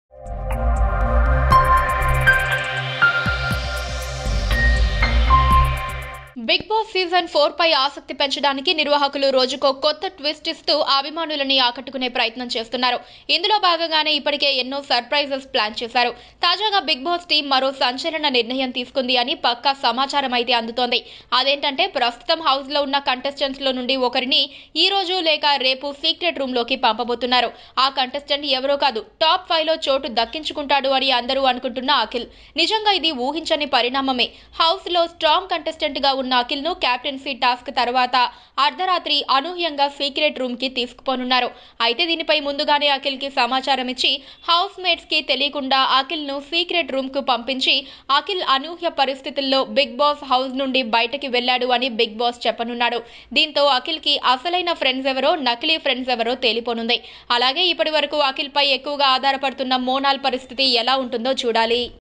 Big Twist Is BiggBossHouse | Is Akhil Going To Secret Room From The House | SahithTv
We are requesting you to please share your thoughts and videos with us to make more creative ...